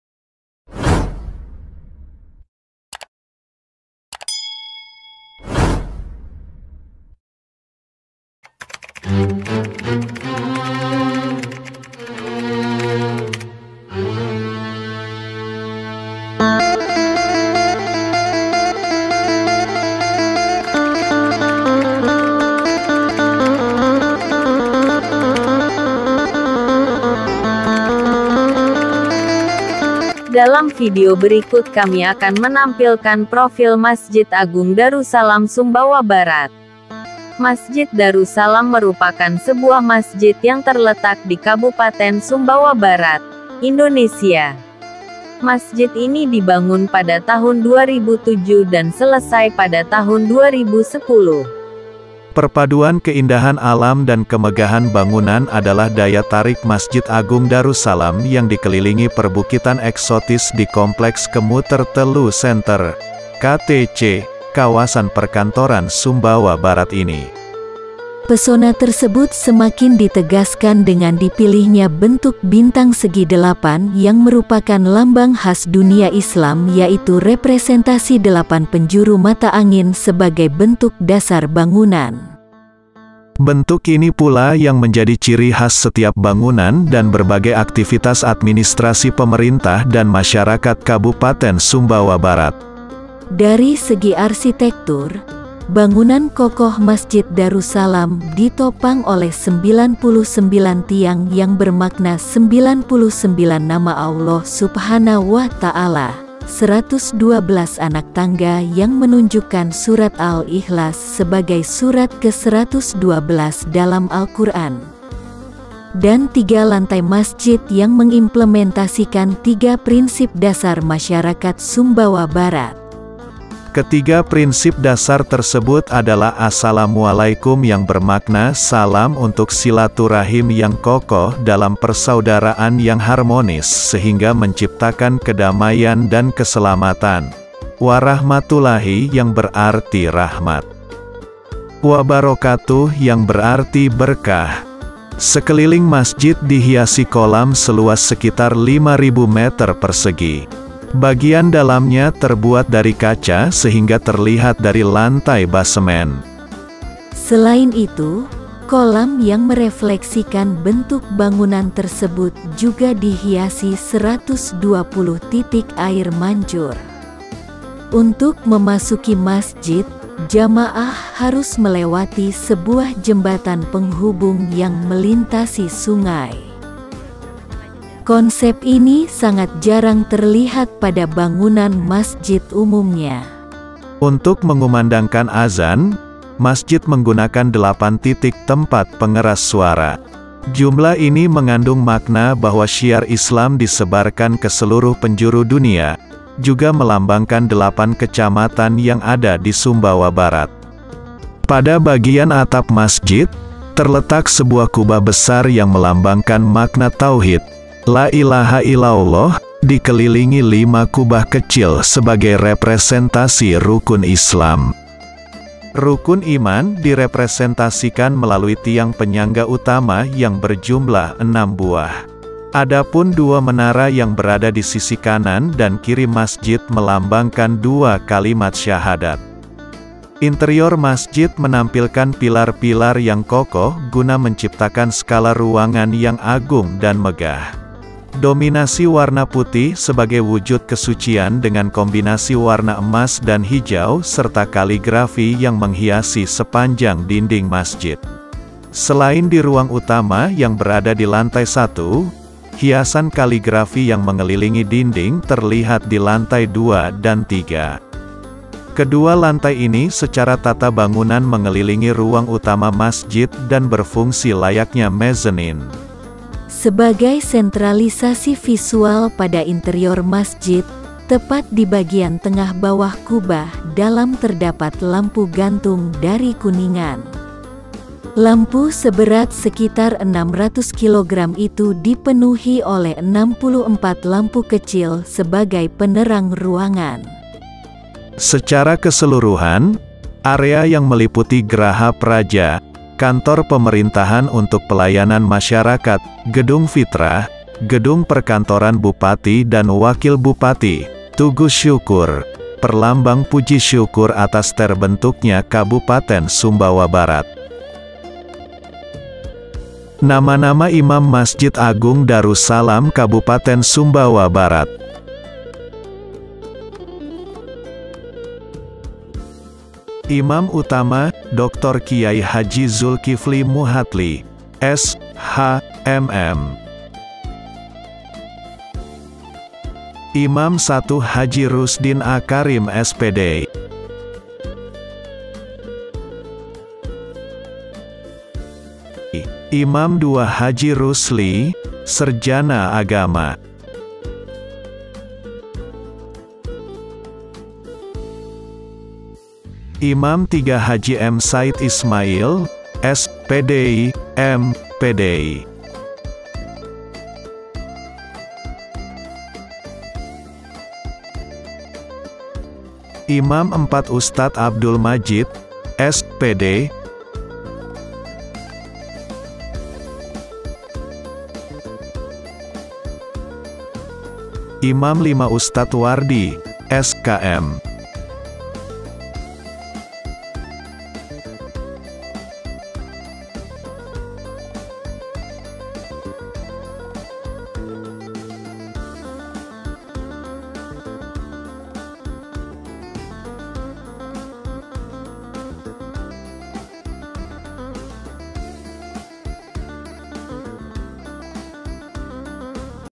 Sejarah Singkat Masjid Agung Darussalam Sumbawa Barat
Dalam video berikut saya coba menampilkan Sejarah sngkat Masjid Agung Darussalam KSB, Masjid terbesar dan salah satu yang termewah di tanah Nusa Tenggara Barat ini, selamat menyimak,semoga Vidio ini bermanfaat yah,jangan lupa like,komen,dan subscribe yah😊😊🙏🙏